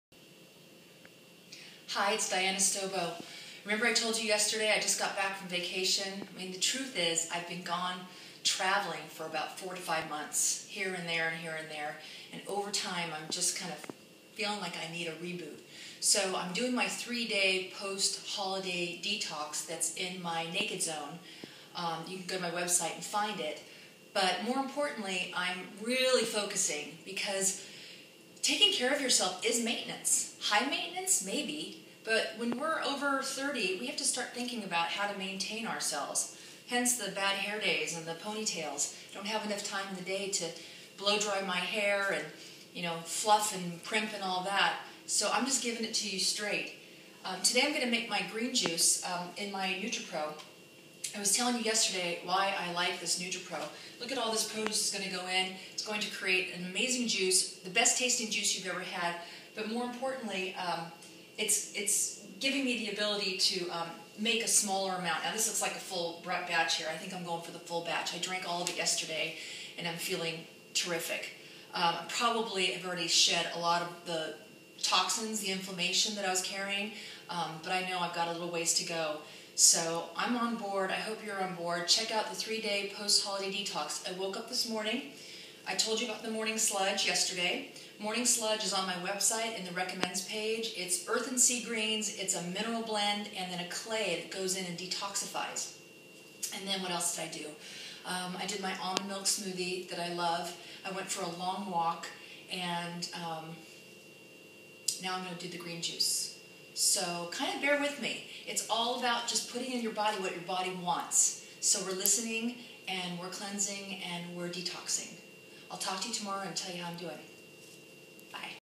Raw food diet 3day post holiday detox
On day 2 of my return home, I begin juicing greens along with my morning sludge.  to find out how u can do a quick detox.  And join me